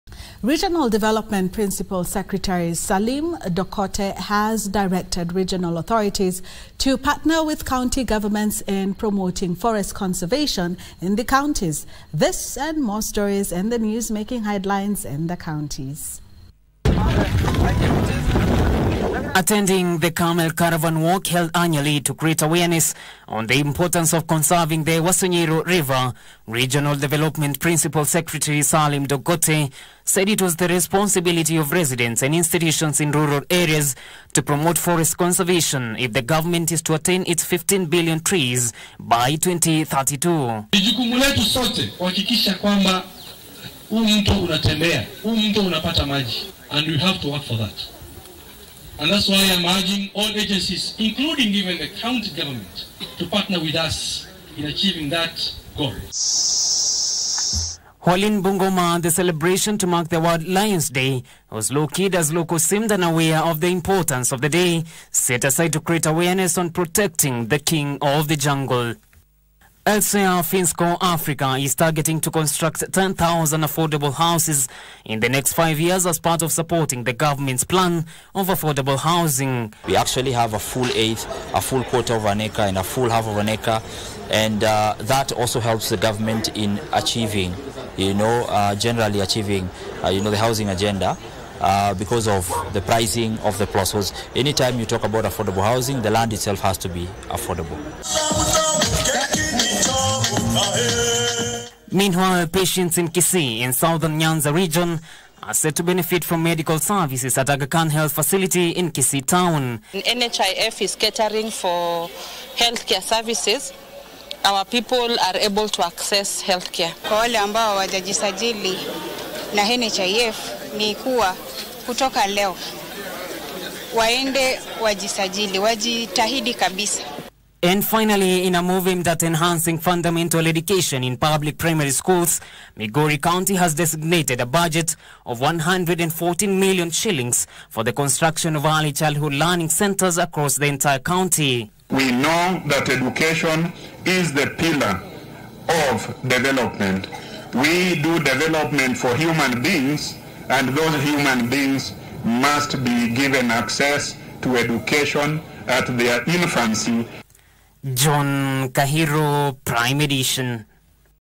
Salim Dokote directs regional authorities, to partner with county governments to conserve forests
Regional Development Principal Secretary, Salim Dokote has directed regional authorities, to partner with county governments in promoting forest conservation in the country.